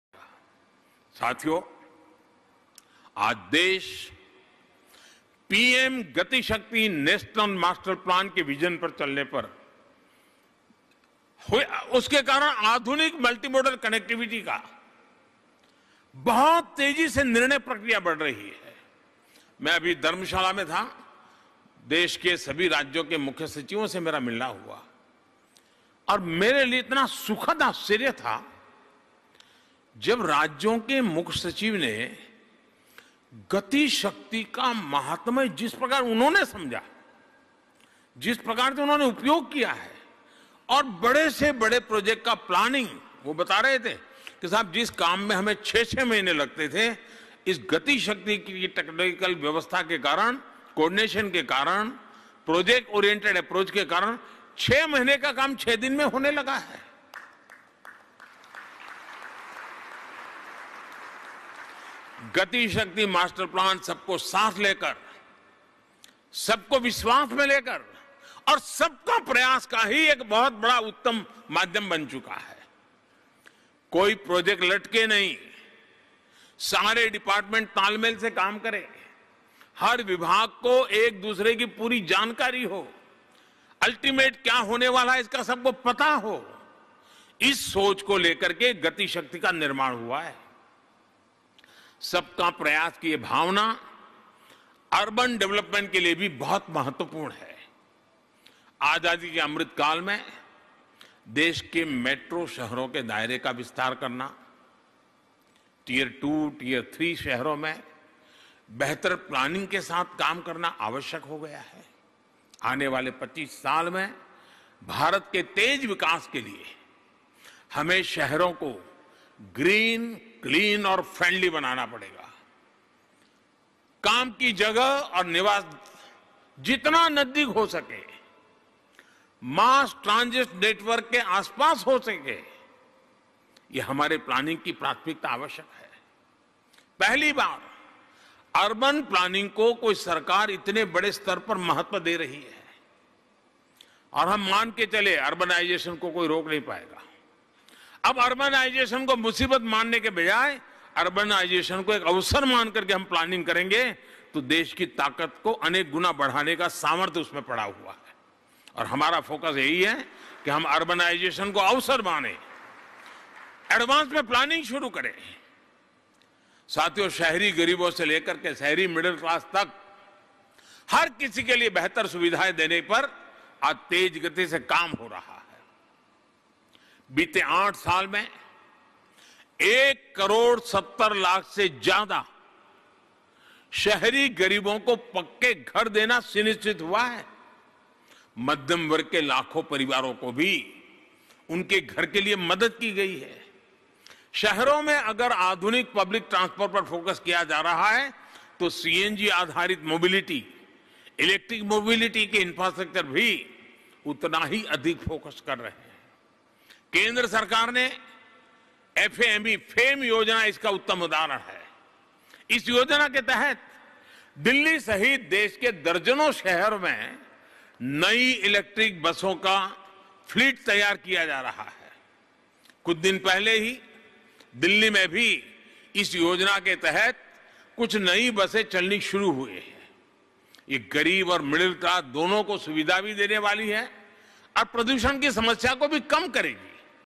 PM Modi Inaugurates Main Tunnel & Underpasses of Pragati Maidan Integrated Transit Corridor Project
In the last 8 years, we have taken unprecedented steps to solve the problems of Delhi-NCR.

Metro service in Delhi-NCR has expanded from 193 km to 400 km in the last 8 years: Prime Minister of India Shri Narendra Modi

Today, Delhi has received a beautiful gift of modern infrastructure from the Central Government: PM Narendra Modi at the inauguration of Pragati Maidan Integrated Transit Corridor

PM Modi dedicates the main tunnel and five underpasses of the PragatiMaidan Integrated Transit Corridor project in Delhi to the nation.

PM Narendra Modi Inaugurates Main Tunnel & Underpasses of Pragati Maidan Integrated Transit Corridor Project